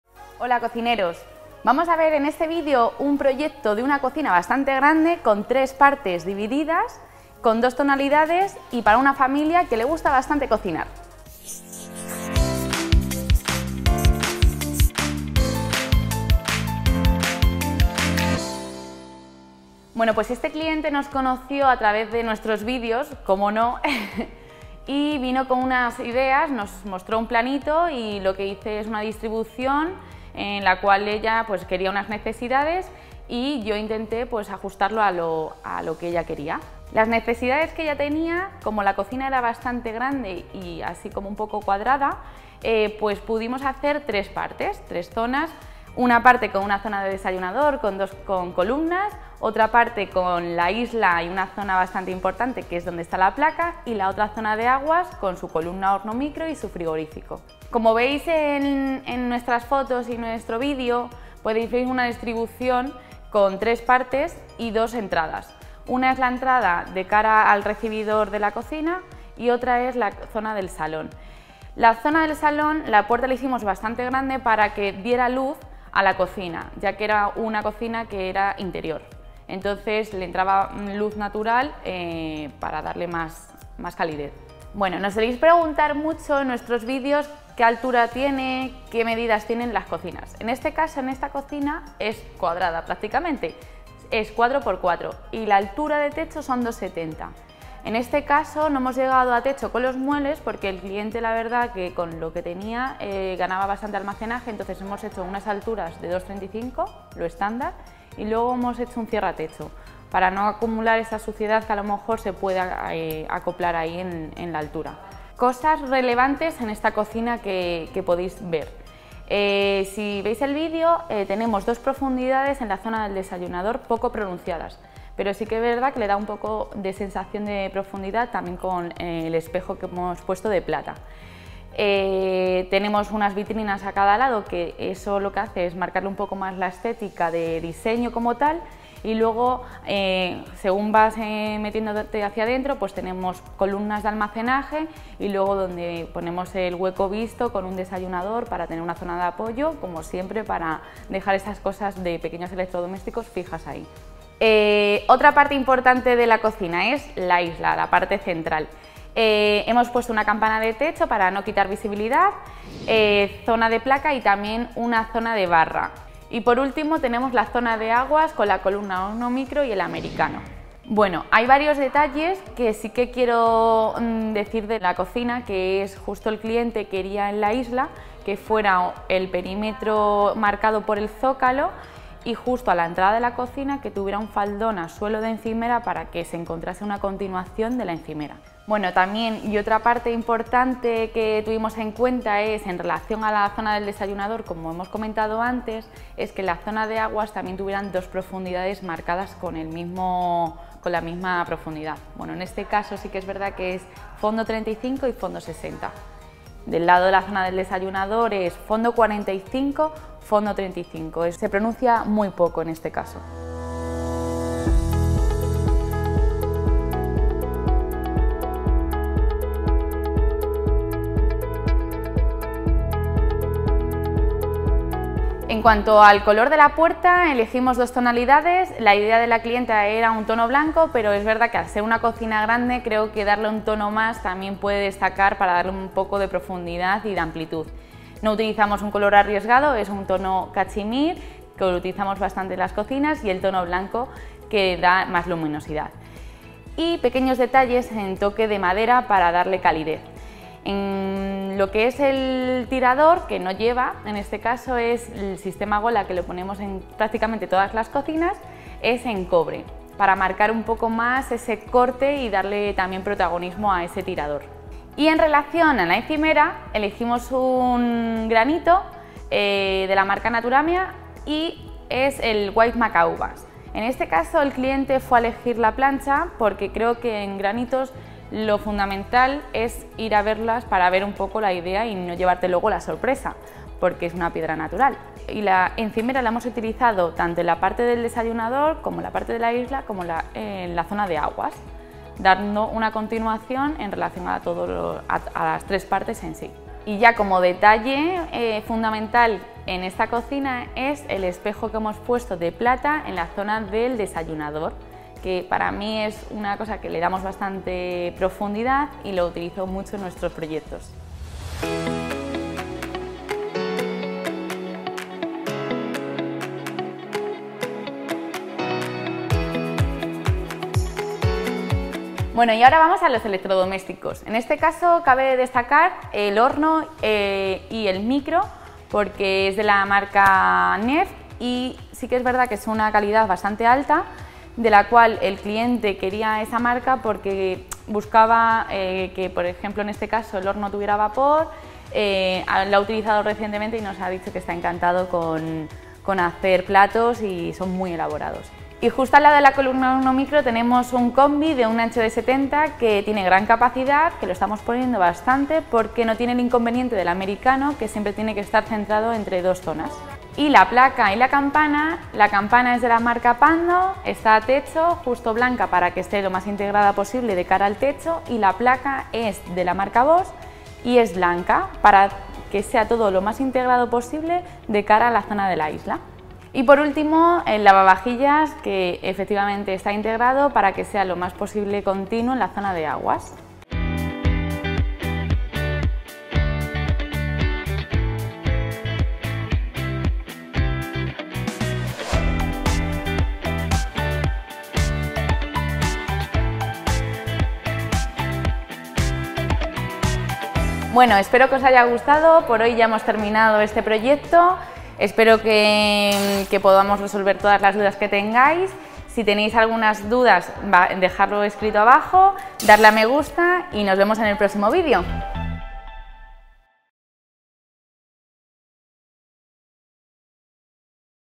Cocina BLANCA con ISLA y DESAYUNADOR
En este vídeo os enseñamos una cocina de 16m2 con forma cuadrada.

Ambos frentes tienen unos 4 metros de longitud y 4 de ancho. La altura es algo más elevada, pero hemos utilizado muebles estándar un cierre a techo.

Lo más singular es la mezcla de colores cashmere y blanca y el desayunador en frontal plata, además de la encimera White Macaubas de Naturamia. Una piedra natural que nos encanta.

¿Te gustaría cambiar tu cocina? ¿Quieres un diseño de Línea 3 Cocinas? Ahora te lo ponemos mucho más fácil gracias a nuestros PACKS DE DISEÑO. ¡Consulta toda la información en nuestra web!